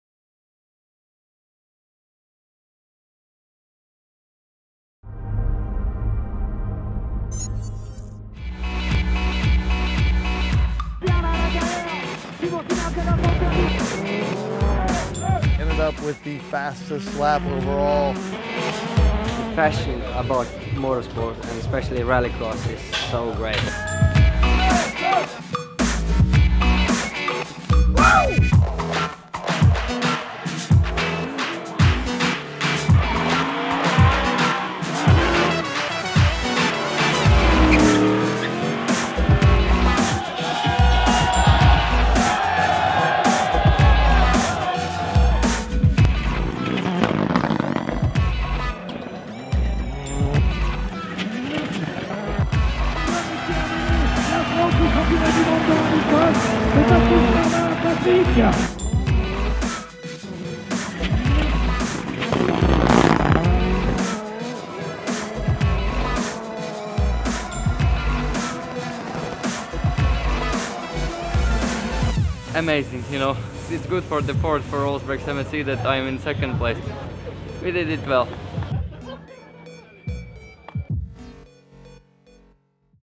Ford Fiesta and Mustang Tear Around France | FIA World Rallycross | Ford Performance
Reinis Nitišs put Olsbergs MSE back on the FIA World Rallycross Championship podium in France, Ken Block finishes 4th and Vaughn Gittin Jr. shows off Ford Mustang muscle

About FIA World Rallycross Championship:
The FIA World Rallycross Championship is fast, competitive and spectacular. Watch for all the four-wheel racing elements you love.